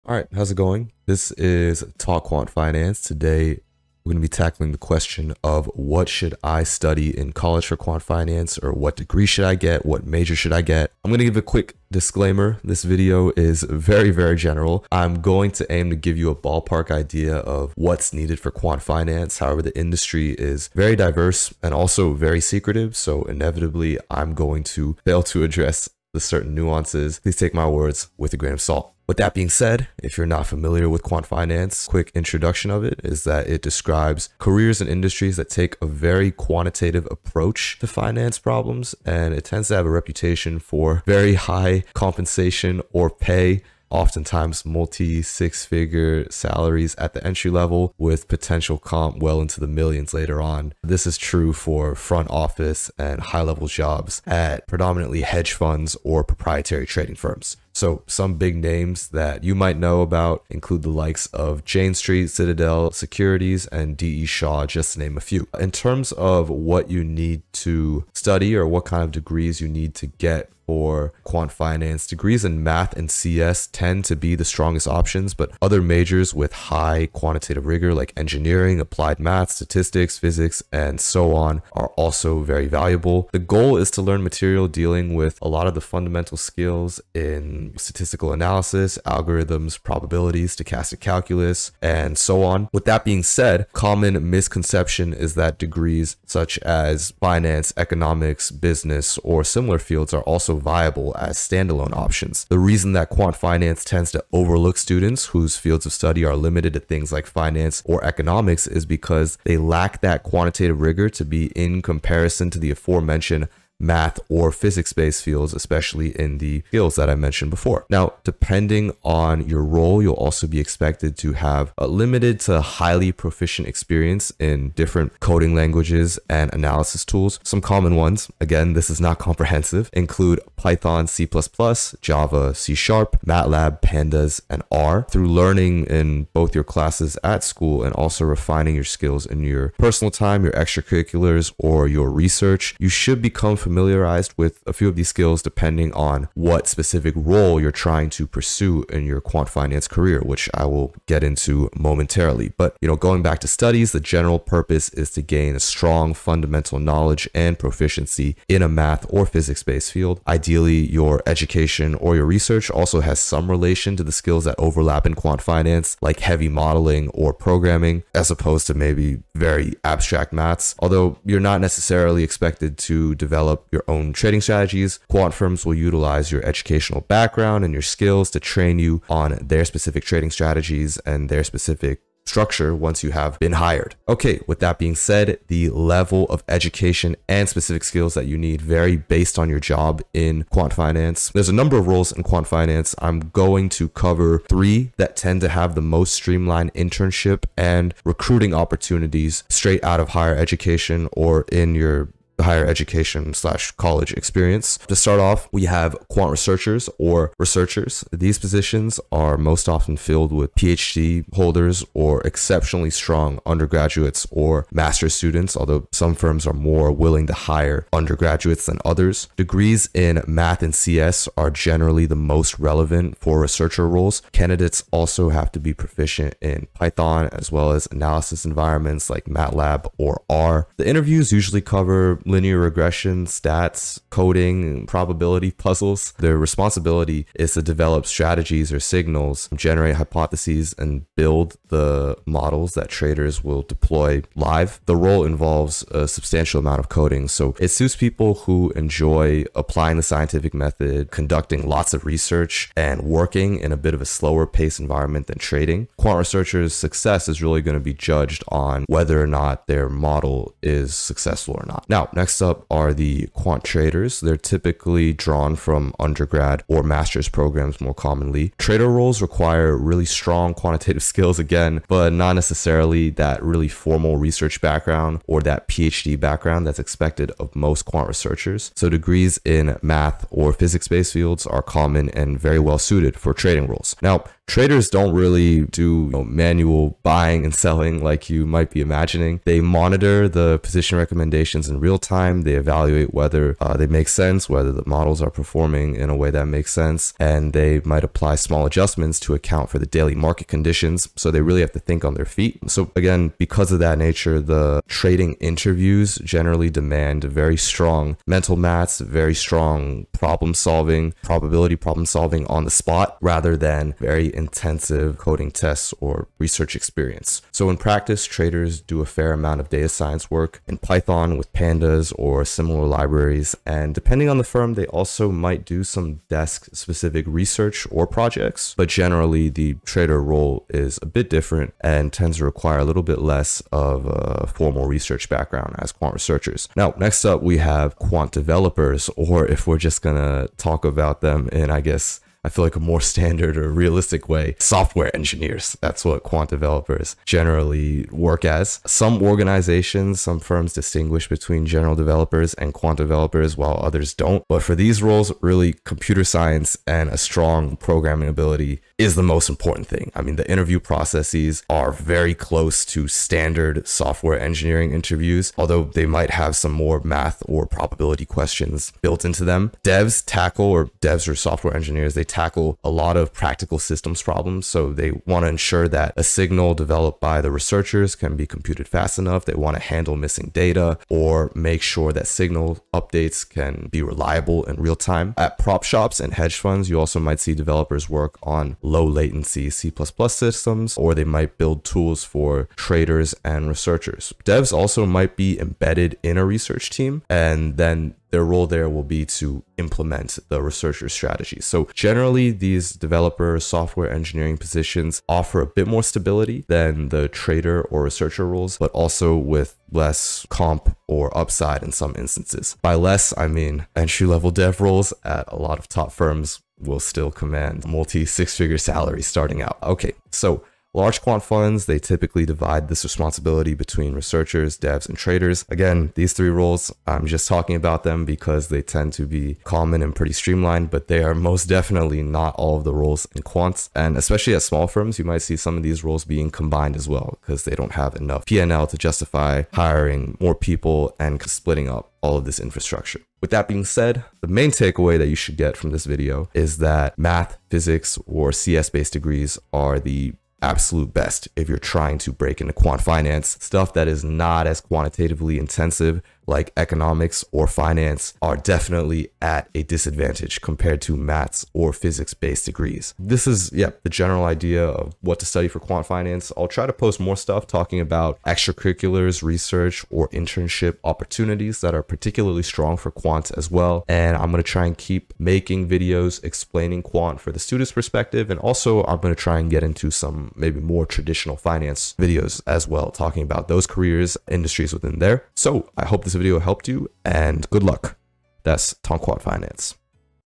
What Should I Study in College for Quant Finance? (Quant Degrees & Majors)
===Timestamps===

0:00 What to Study for Quant Finance? (Disclaimer)
0:30 Intro to Quant Finance
1:05 College Majors/Degrees
1:35 Finance/Economics Degrees?
1:58 Coding Languages/Analysis Tools to Know
2:33 The Role of Your Education in Quant
3:05 Education Focus for Different Quant Jobs
3:26 Education for Quant Researchers
4:23 Education for Quant Traders
5:37 Education for Quant Developers/SWE
7:00 Division of Primary Quant Roles
7:23 Main Takeaways

===End===